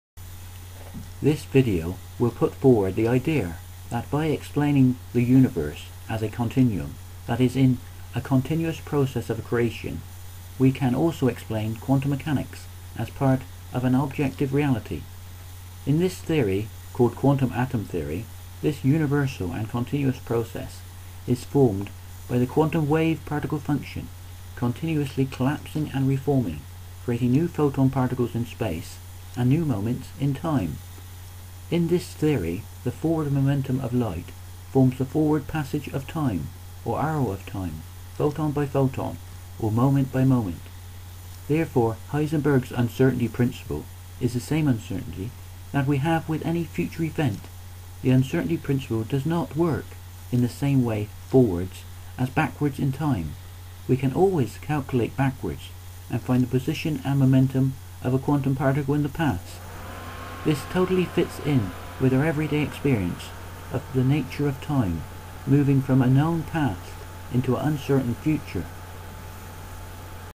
Can Quantum Mechanics have an objective reality?
This video will put forward the idea that by explaining the Universe   as a continuum that is in a continuous process of creation, we can also explain Quantum mechanics as part of an objective reality.

In this theory called Quantum Atom Theory this universal and continuous process is formed by the quantum wave particle function.

  Continuously collapsing and reforming creating new photon particles in space and new moments in time.

In this theory the forward momentum of light forms the forward passage of time or arrow of time photon by photon or moment by moment.

Therefore Heisenberg's Uncertainty Principle is the same uncertainty that we have with any future event. The Uncertainty Principle does not work in the same way forwards and backwards in time.

We can always calculate backwards and find the position and momentum of a quantum particle in the past.

This totally fits in with our everyday experience of the nature of time, moving from a known past into an uncertain future.